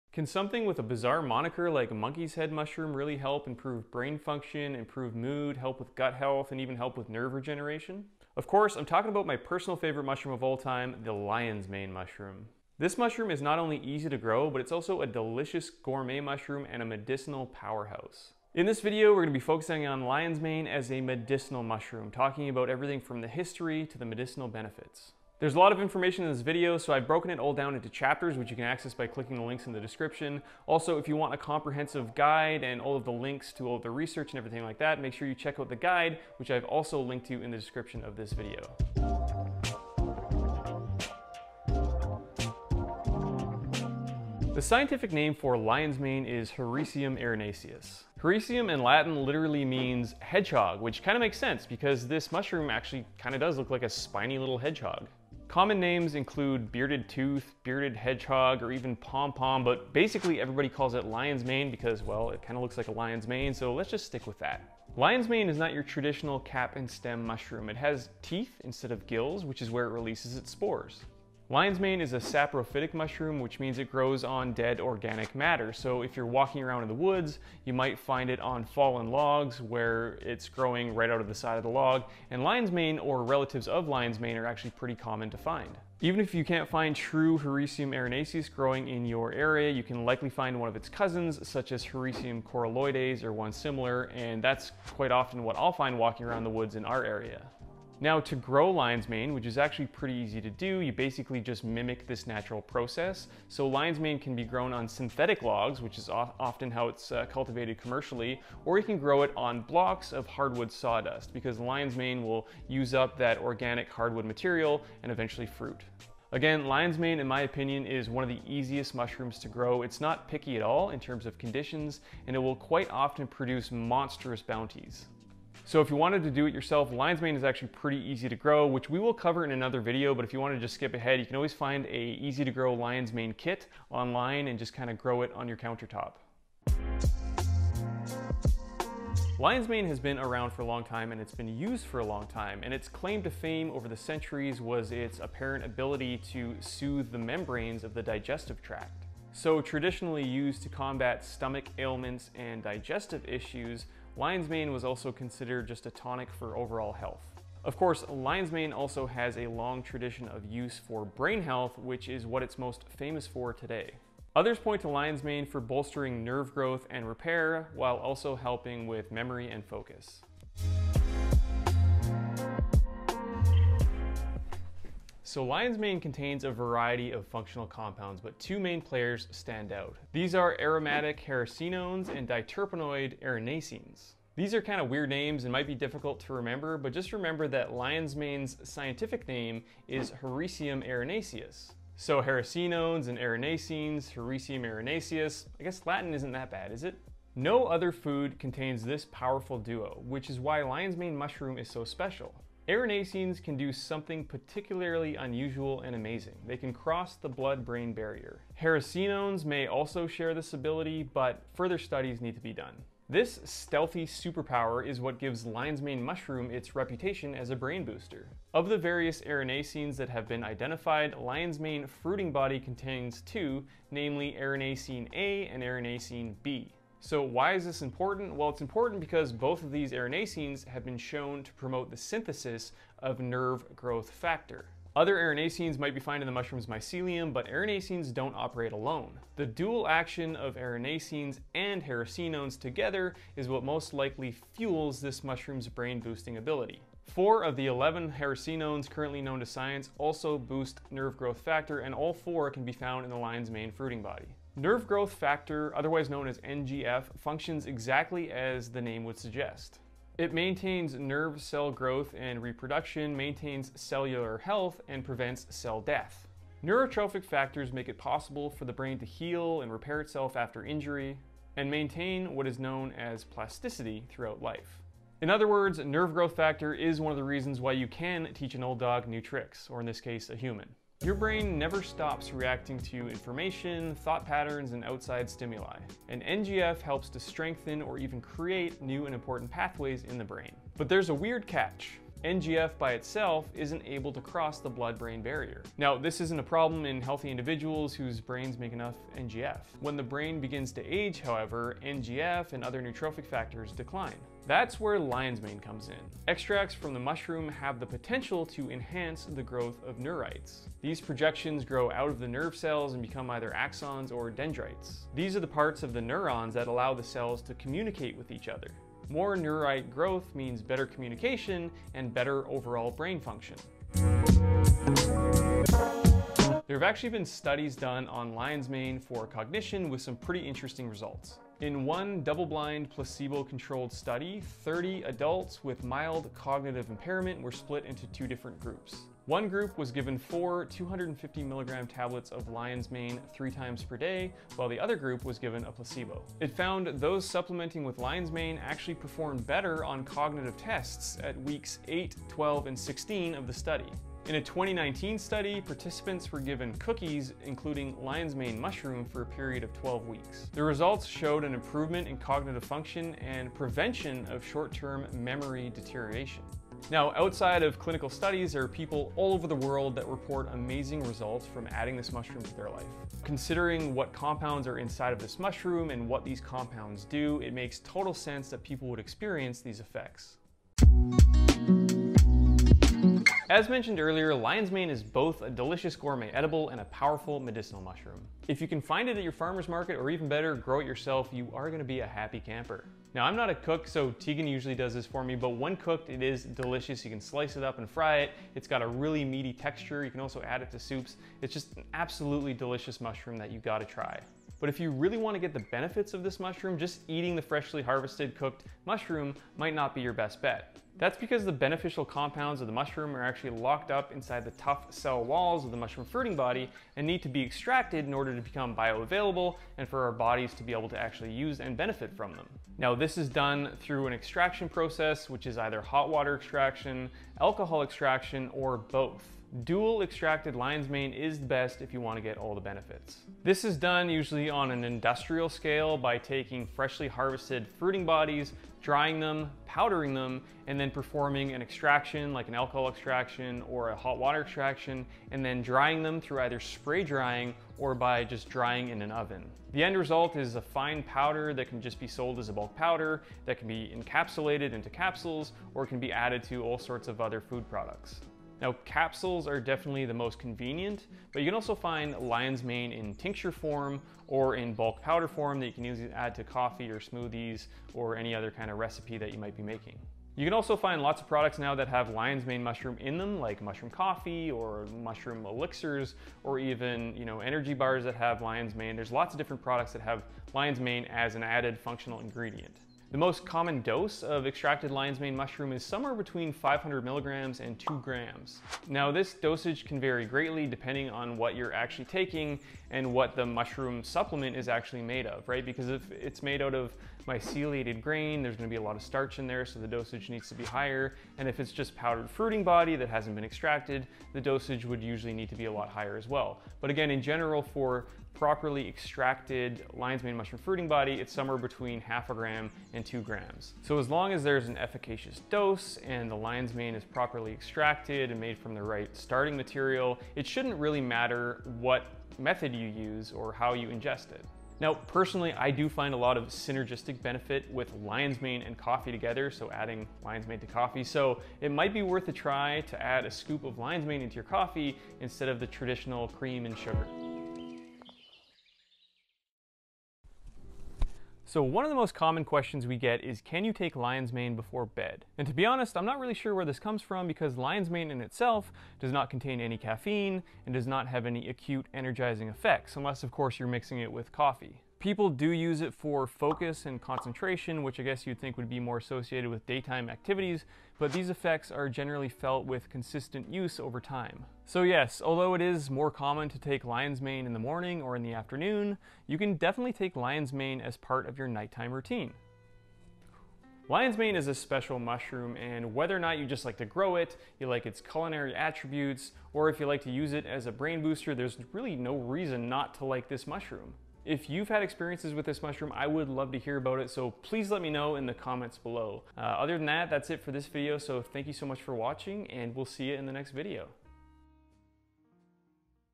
Brain Booster? The Magic Power Of Lion's Mane Mushroom (aka Hericium erinaceus)
Explore the power of Lion's Mane mushroom—known scientifically as Hericium erinaceus—focusing on benefits, uses and how to incorporate it into your routine.

You'll learn the traditional uses of lionsmanemushroom, its unique active compounds (hericenones and erinacines) and why biohackers love its effects.

Want to read the full guide?

0:00 Intro
0:43 What Is Lion's Mane
2:16 Traditional Uses
2:51 How It Works
5:25 Studies On Lion's Mane and Cognition
6:23 Best Way To Use
8:57 Lion's Mane Before Bed?
9:32 Outro

🦁 Try premium Lion's Mane for yourself!